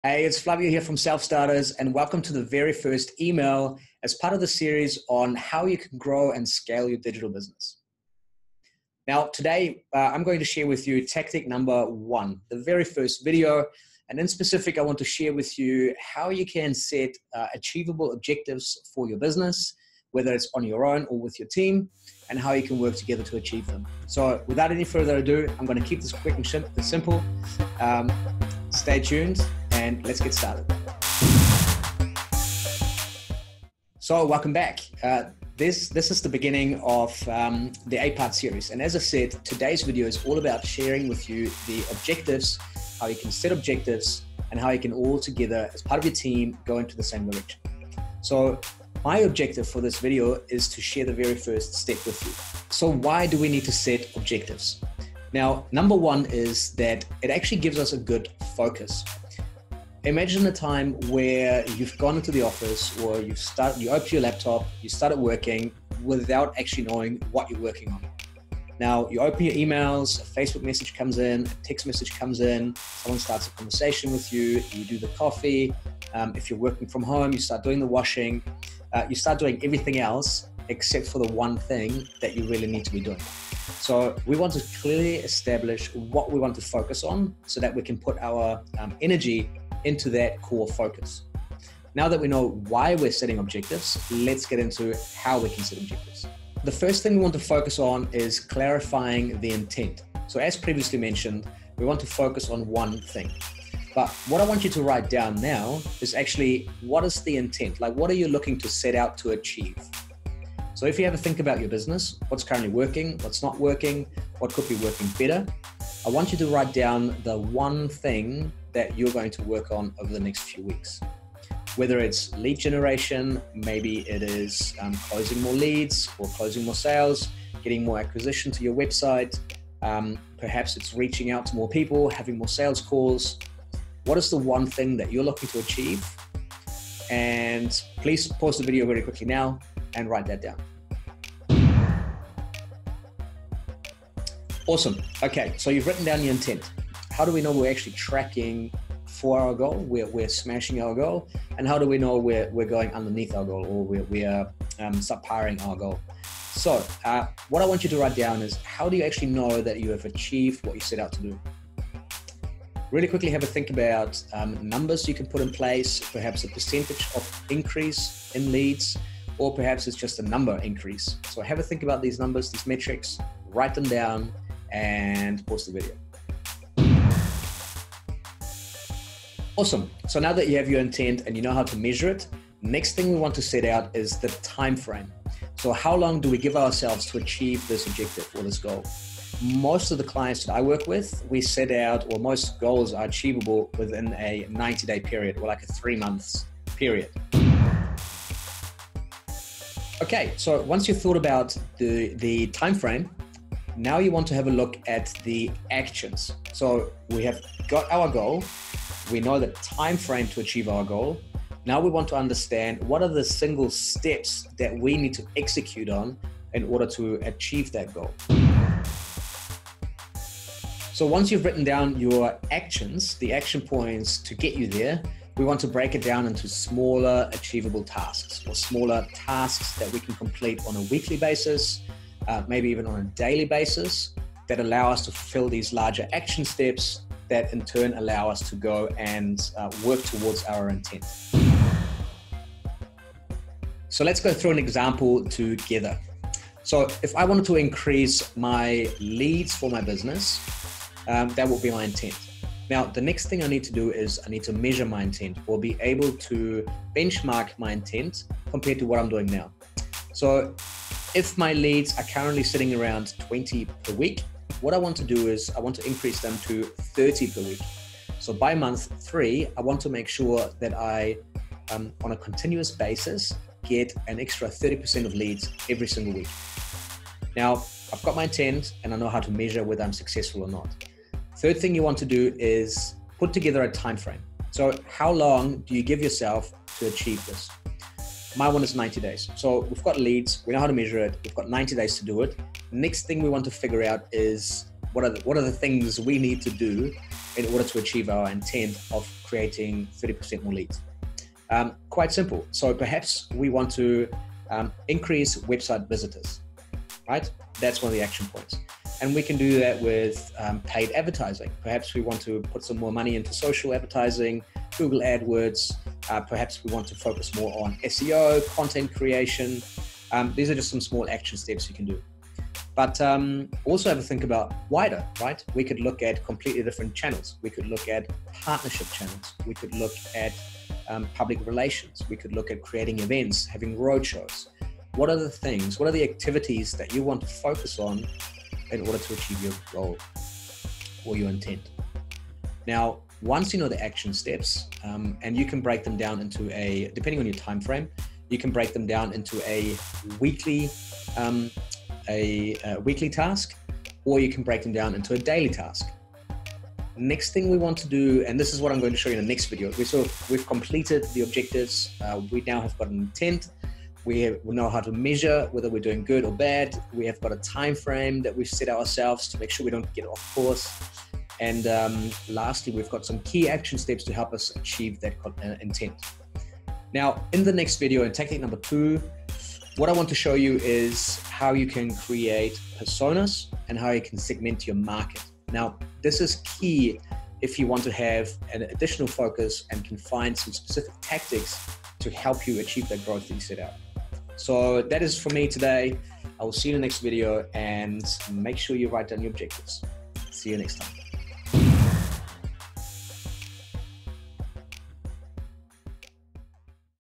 Business Growth tactic #1 - Setting Objectives
Understanding what you intent to achieve in your business will enable you to move into a direction and measure your results against some benchmarks that’ll tell you whether you are on track to achieve your goal or shooting off-piste.

So where do you start?

The first part you want to identify is what the intent you look to achieve is. And depending on what stage of business you are in, this could vary. Perhaps you target higher customer acquisition, look to increase revenue or become better at closing leads.

Whatever the result you want to hit, you must ensure that you put in place metrics. To help you with metrics, it is like thinking about how you will actually know that you have achieved your goal. Maybe you measure the amount of closed sales, perhaps the quantity of people visiting your site or the amount of repeat customers or lifetime value of a customer you have.

Once you know the outcome and how to measure its success, you set out to create timeframe in which you want to achieve this, which in turn gives you the length of your roadmap. From here you should plan your actions on a monthly, weekly or even daily basis.

This will give you a framework of what you are looking to achieve, how to measure it, over what period and what actions you need to accomplish in order to get to your desired outcome.

The key takeaway here is that if you can’t measure it, you cannot manage it and therefore not grow it. A clearly defined outcome and some a way to measure it is key here.

You should look at achieving your goals based on data rather than luck. Here’s a list of things to consider when planning your objectives:

What is the desired outcome you look to achieve and how will you know you achieved it
What is the timeframe in which you look to achieve your goal (think about your start and finish dates
What actions can you describe that will help you achieve your weekly tasks